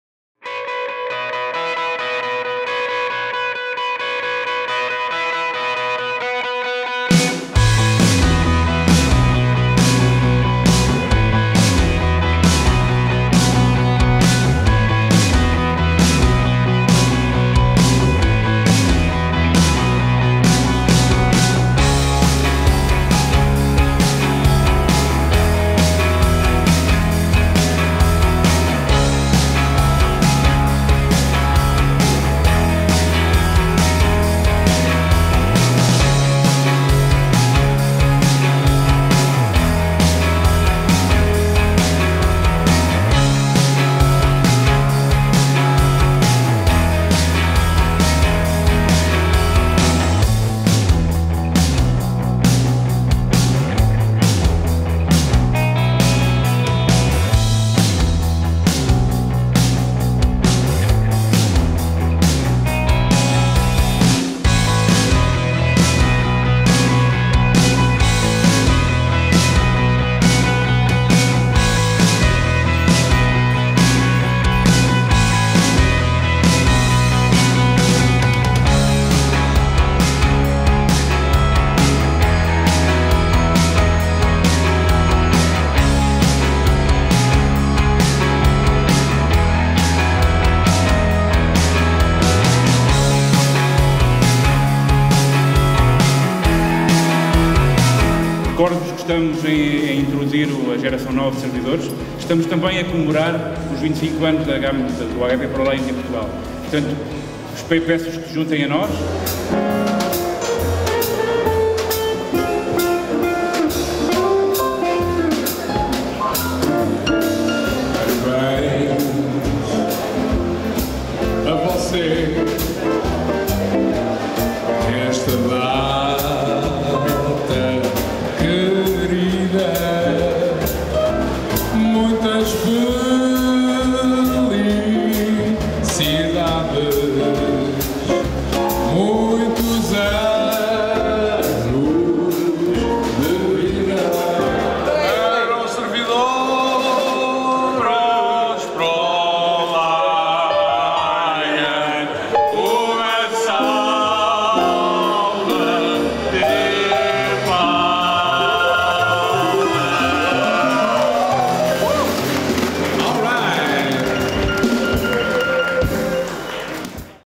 HP SERVER INNOVATION DAY - 25 de Setembro 2014 em Lisboa, Museu da Carris
A HP e a Intel reuniram mais de 270 profissionais de TI para revelar as tecnologias invovadoras dos novos servidores HP ProLiant Gen9.